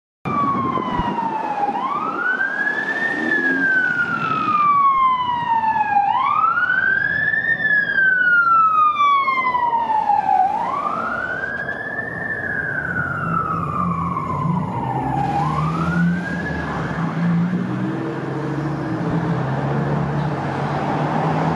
Toronto Fire HAZ145 Responding
Toronto Fire Services Hazmat 145 responding for a natural gas fire in 311s area